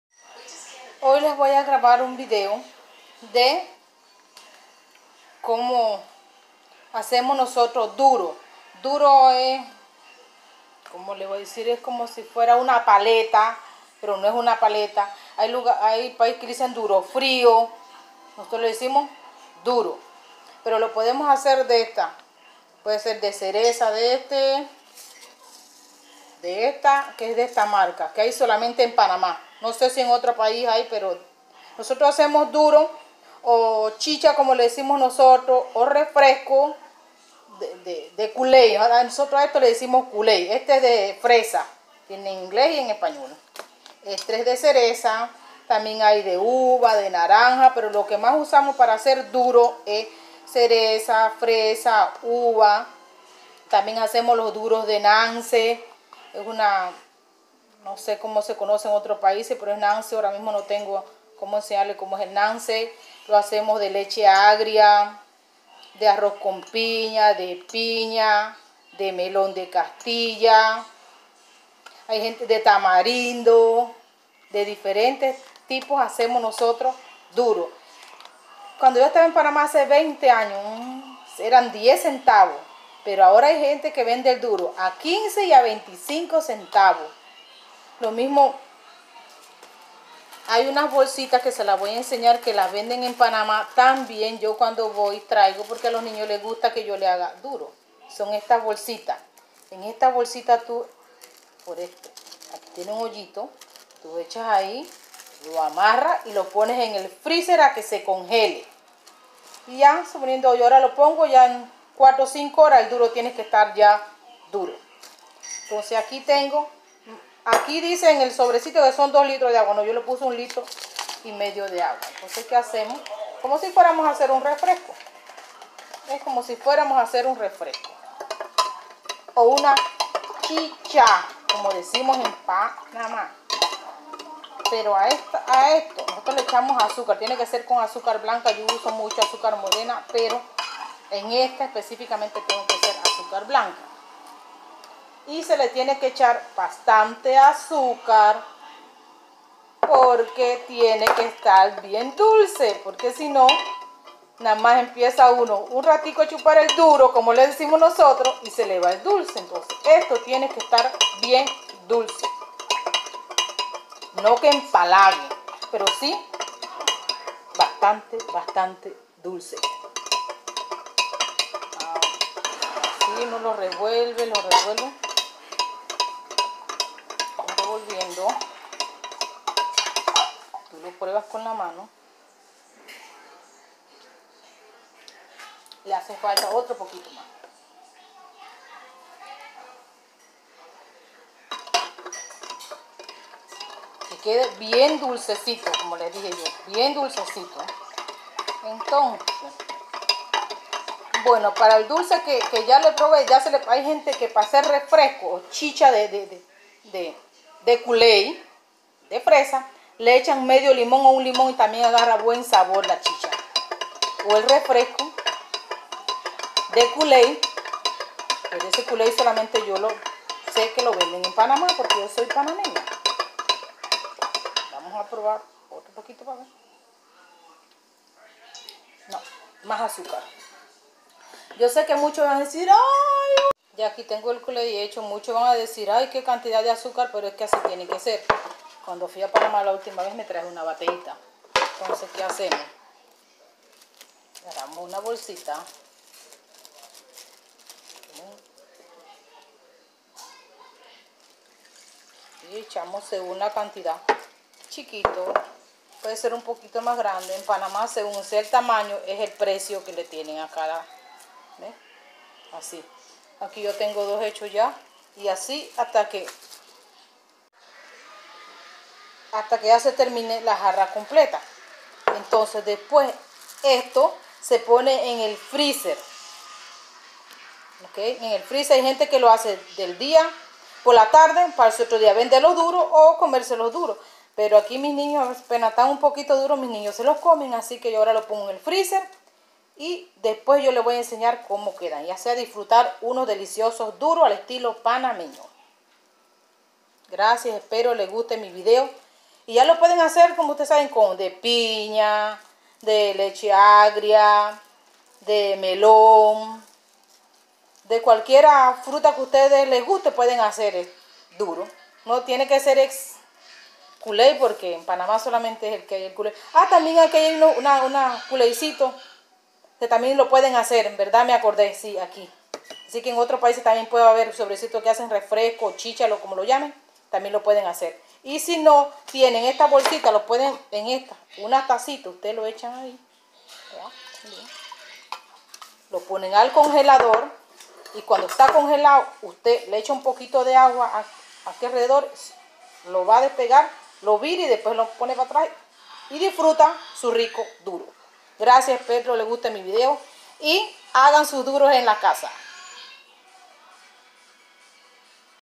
Duro al estilo Panameño
En muy refrescante y lo venden en Panamá y se puede hacer de muchas frutas oh otros sabores de refresco.